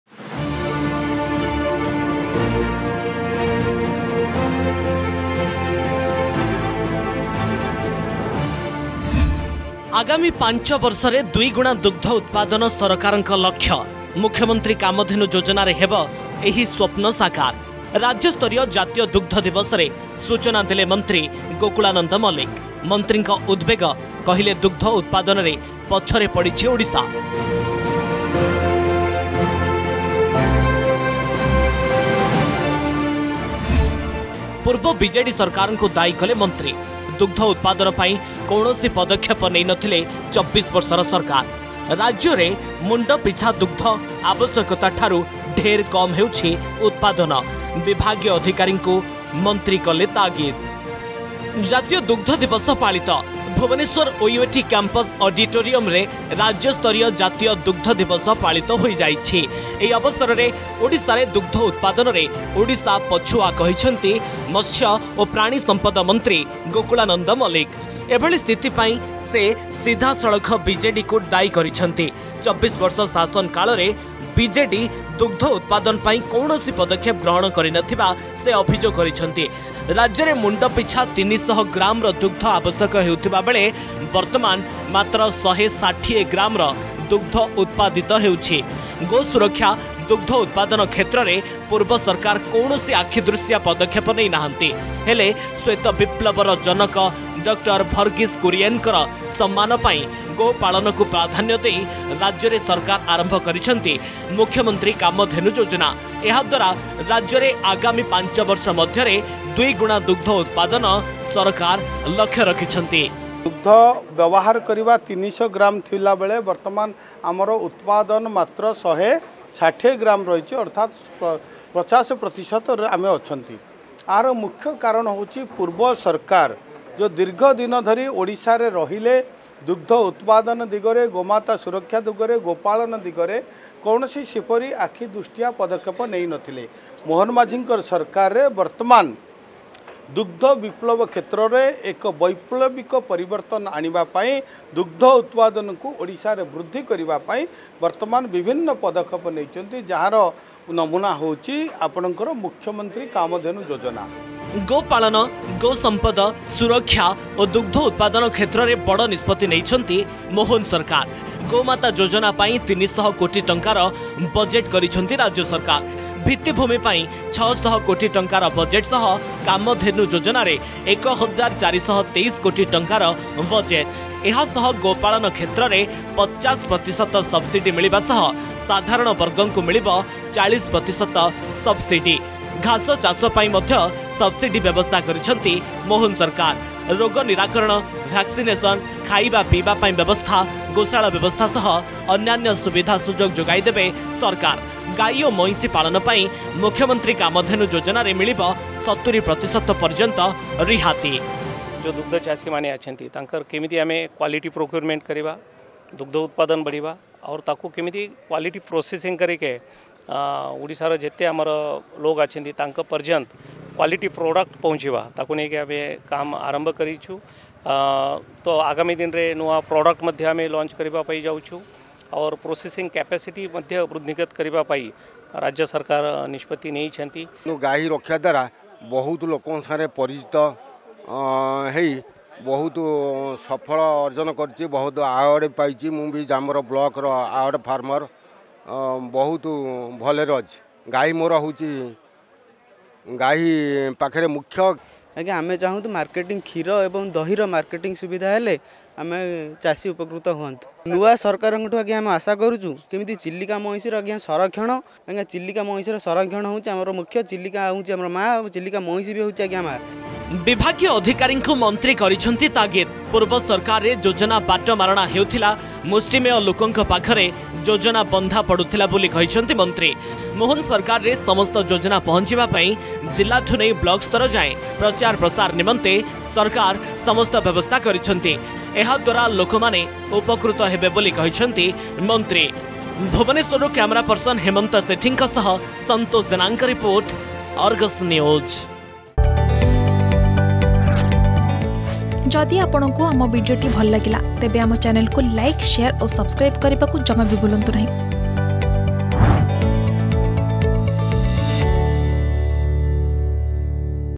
ଆଗାମୀ ୫ବର୍ଷରେ ଦୁଇ ଗୁଣା ଦୁଗ୍ଧ ଉତ୍ପାଦନ ସରକାରଙ୍କ ଲକ୍ଷ୍ୟ | Milk Production | Odisha Govt
ଆଗାମୀ ୫ବର୍ଷରେ ଦୁଇ ଗୁଣା ଦୁଗ୍ଧ ଉତ୍ପାଦନ ଲକ୍ଷ୍ୟରେ ରାଜ୍ୟ ସରକାର । ମୁଖ୍ୟମନ୍ତ୍ରୀ କାମଧେନୁ ଯୋଜନାରେ ହେବ ଏହି ସ୍ବପ୍ନ ସାକାର । ରାଜ୍ୟସ୍ତରୀୟ ଜାତୀୟ ଦୁଗ୍ଧ ଦିବସରେ ସୂଚନା ଦେଲେ ମନ୍ତ୍ରୀ ଗୋକୁଳାନନ୍ଦ ମଲ୍ଲିକ ।